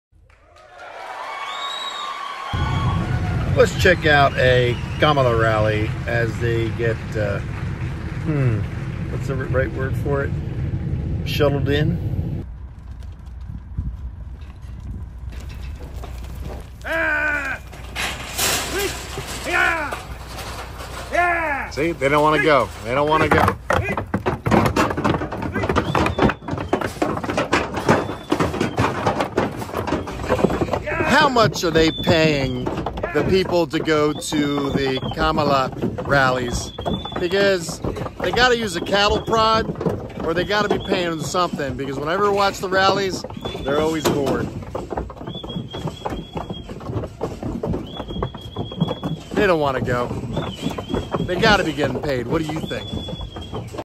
Kamala Harris supporters, why do they go to the rallies?
Things that make you go hmmmmmmmm

I am your Political Commentator & Journalist. Support the Cause. MustangMedicMedia.com